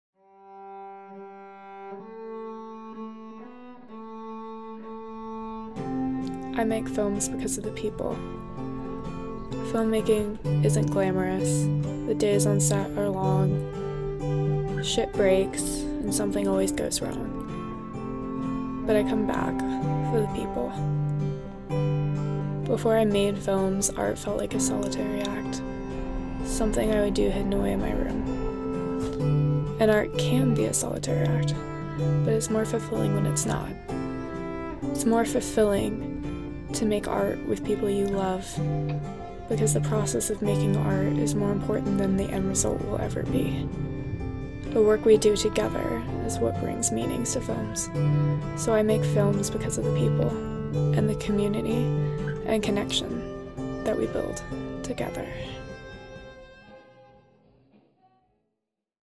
i make films because of the people - video essay
A compilation of camcorder footage from set Summer '23.

Shot by the Dawn Makers Films crew ... Compiled and edited by Bella C Sonen ... Music by Caprie and Lydia Watson